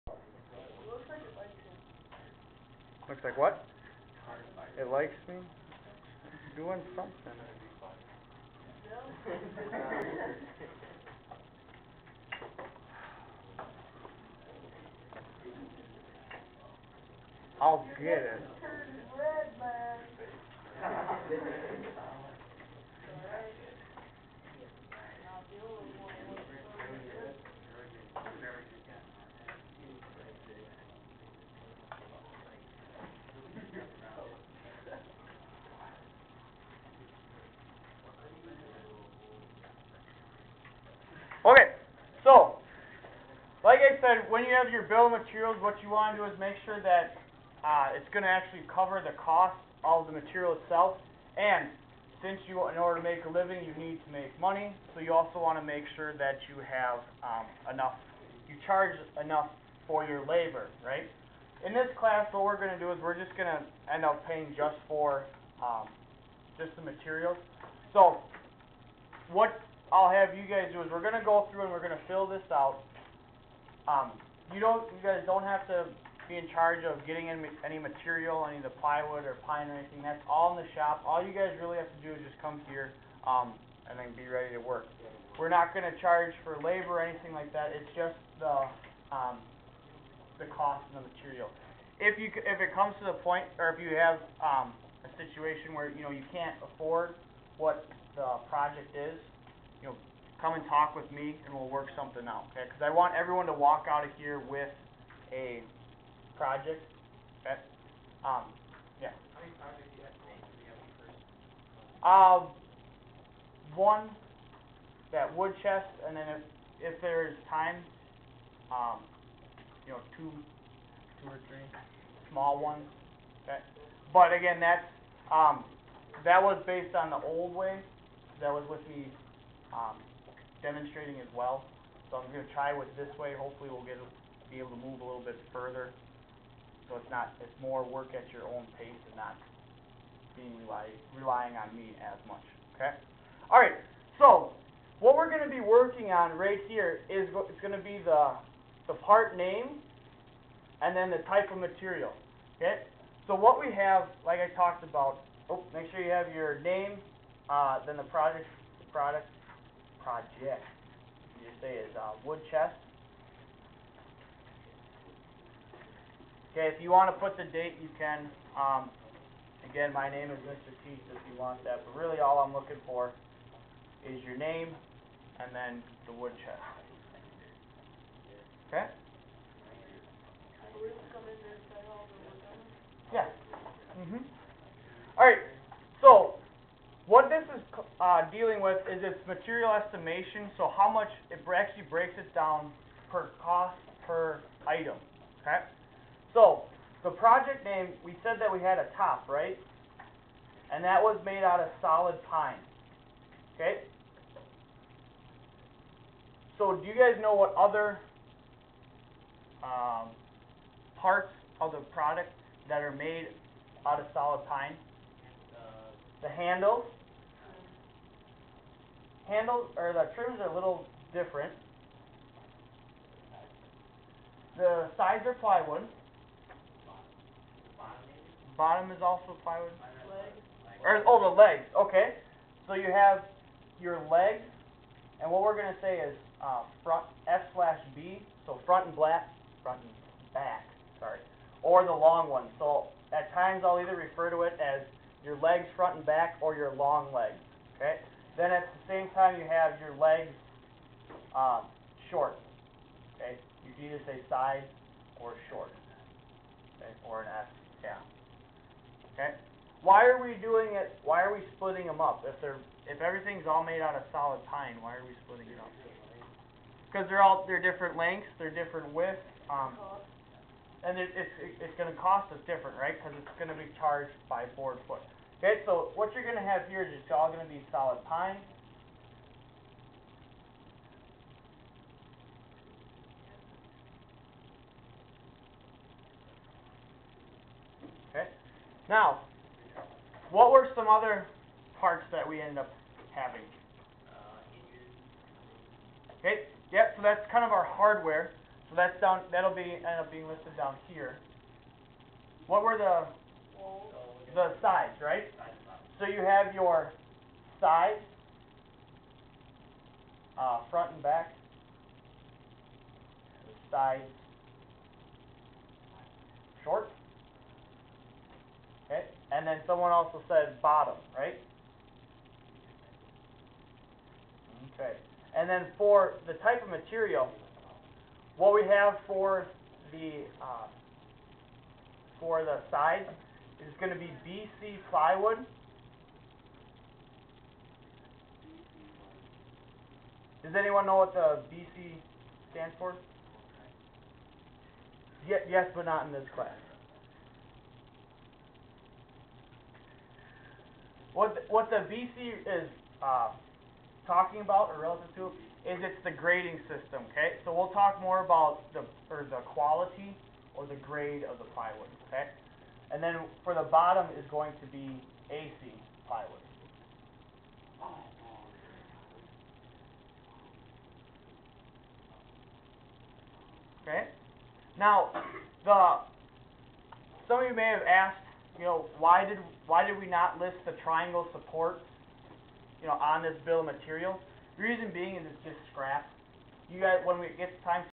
02_Part_3_Woods_1_Filling_Out_The_Bill_Of_Materials
We have a Bill of Materials so we know how much the project is going to cost (This is just for material). We need to list all the pieces that are different sizes so we know exactly how much a piece is going to cost just for the material. Fill out the columns; Part name, material type, and quantity.